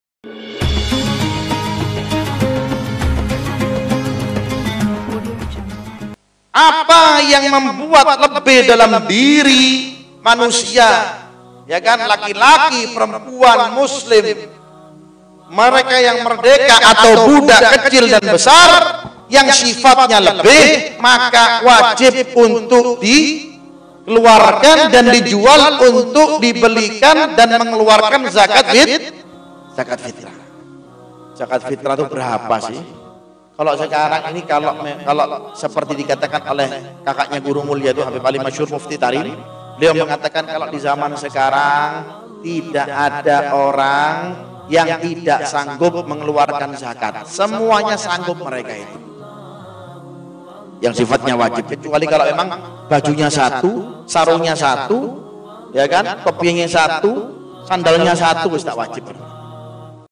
Zakat Fitrah, Siapakah Yang Wajib Membayar? (Halaqatur Rasul, 2018) ᴴᴰ | Al Habib Idrus Alaydrus
"Zakat Fitrah, Siapakah Yang Wajib Membayar? Selengkapnya cek video berikut oleh Al Habib Idrus bin Muhammad Al Aydrus"

InsyaAllah Berkah..

majelisrasulullahjawatimur